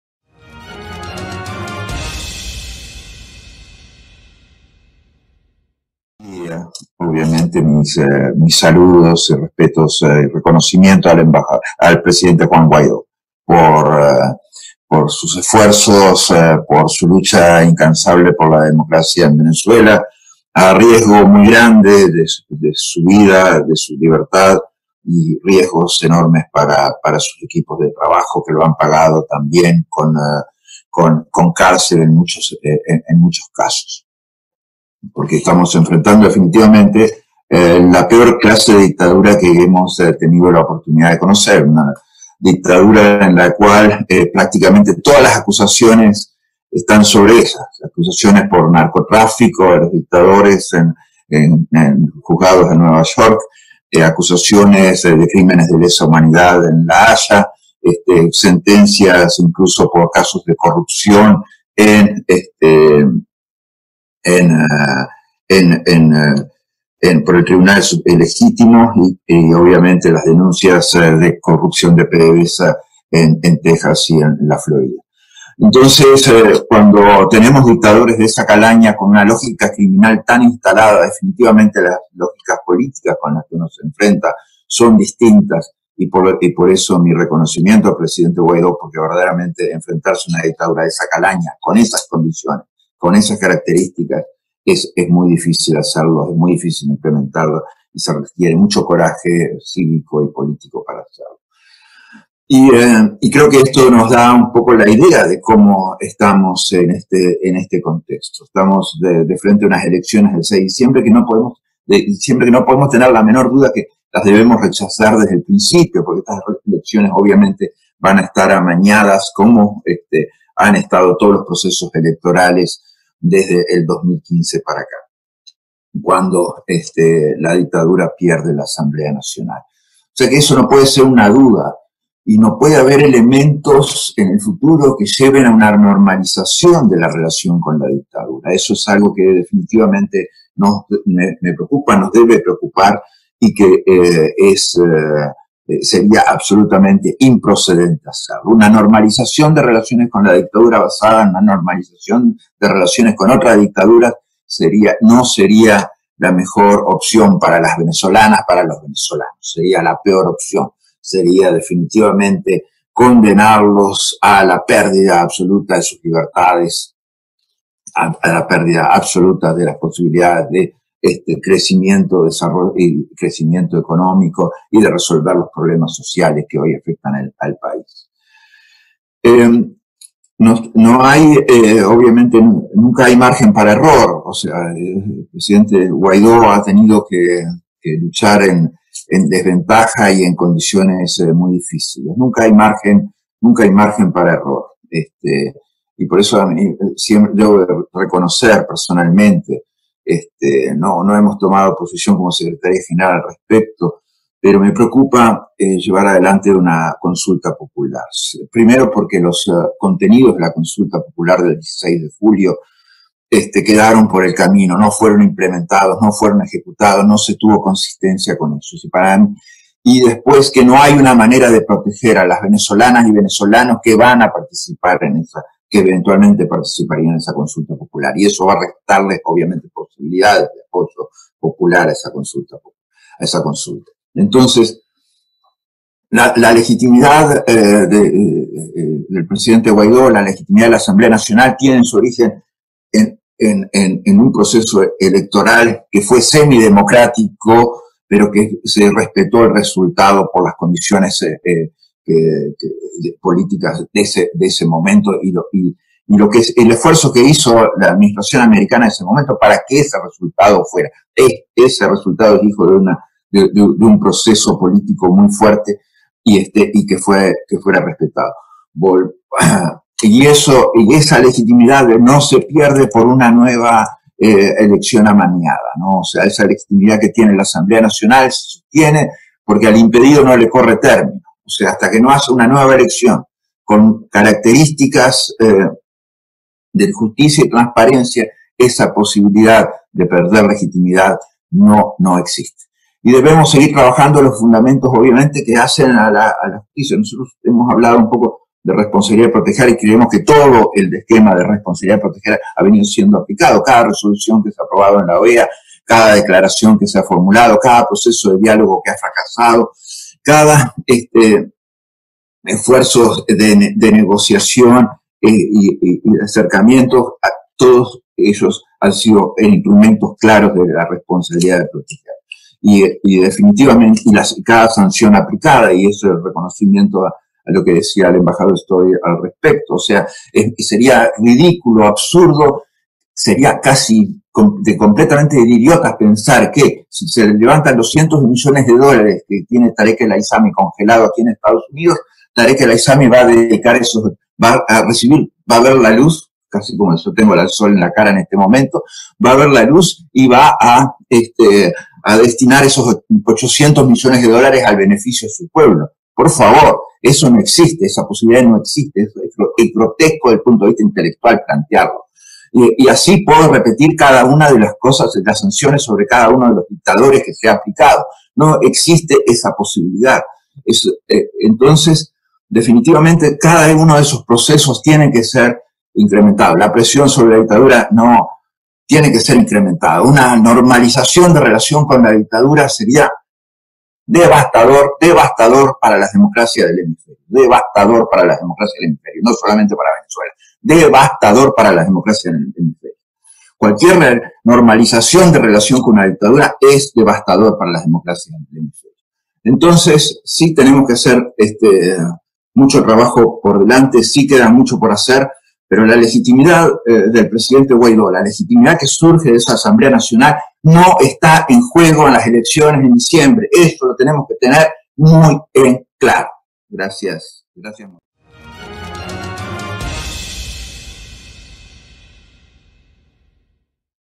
RECHAZO A ELECCIONES DEL 6 DE DICIEMBRE 19NOV IMPORTANTE Luis Almagro
La elección del 6-dic en Venezuela no pone en juego legitimidad de la actual AN ni de jguaido. Normalizar relaciones con la dictadura de Venezuela sería devastador para democracias en las Américas.